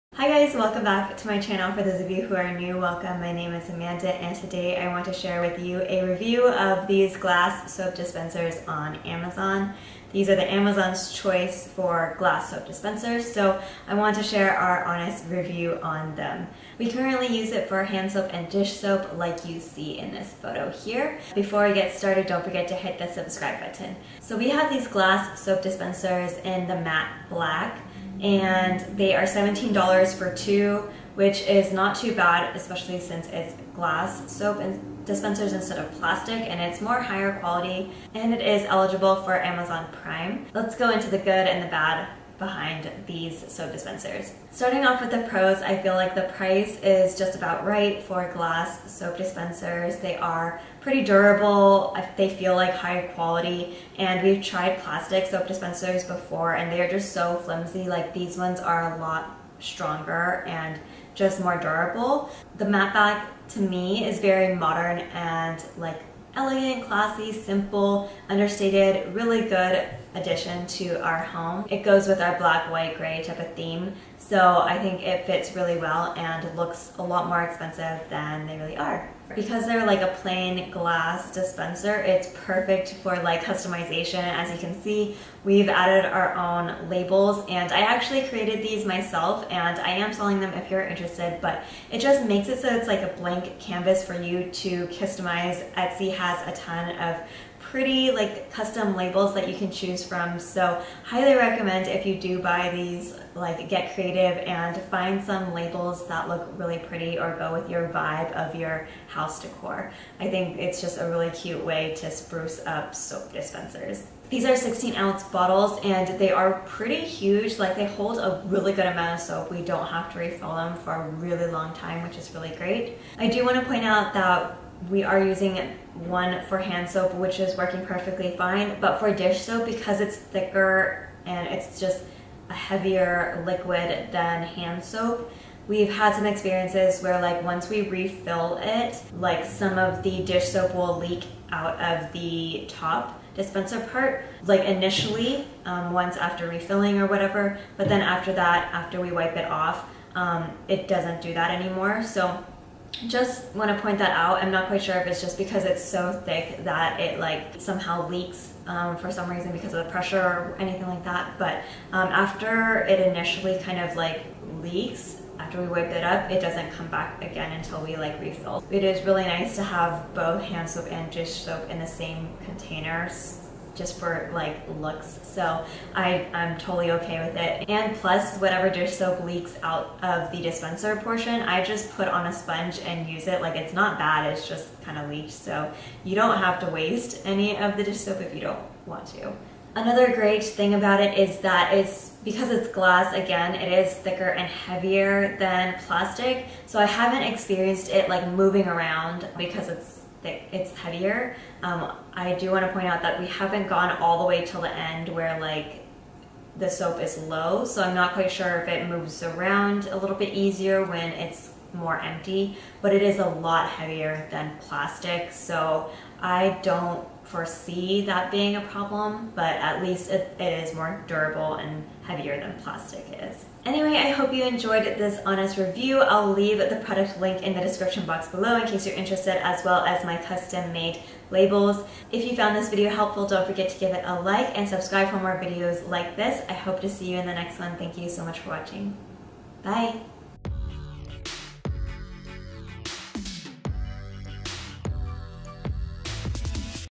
GLASS SOAP DISPENSER REVIEW | AMAZON'S CHOICE
I'm sharing my review of the glass soap dispensers we decided to buy for our home. Enjoy!

Soap Dispensers on Amazon

Hands/Dishes Soap Labels (currently sold out)

LIKE if this video helped you.

CAMERA:
// Sony A6000
// Sony 35mm 1.8 APS-C lens